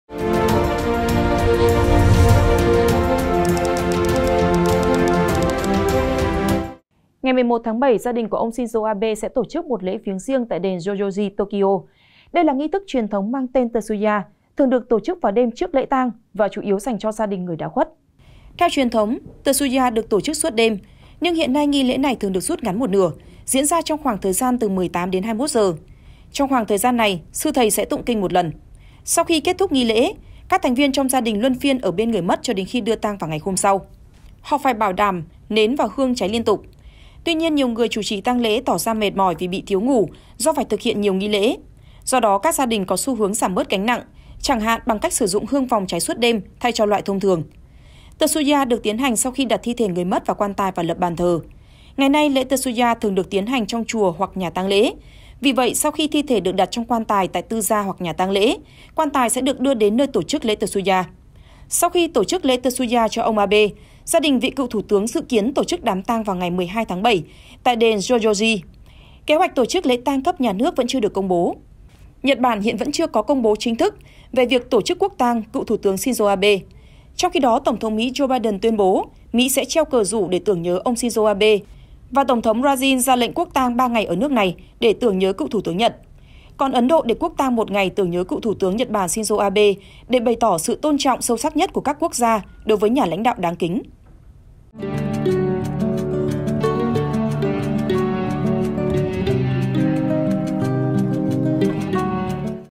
Bật Mí Ý Nghĩa Lễ Tsuya Được Gia Đình Ông Shinzo Abe Thực Hiện Trước Đám Tang | SKĐS
SKĐS | Ngày 11/7, gia đình của ông Shinzo Abe sẽ tổ chức một lễ viếng riêng tại đền Zojoji, Tokyo. Đây là nghi thức truyền thống mang tên Tsuya, thường được tổ chức vào đêm trước lễ tang và chủ yếu dành riêng cho gia đình người đã khuất.

Mời quý vị và các bạn xem thêm: Vì Sao Lễ Tang Cựu Thủ Tướng Shinzo Abe Bị Giới Hạn Khách Viếng Và Làm Đơn Giản?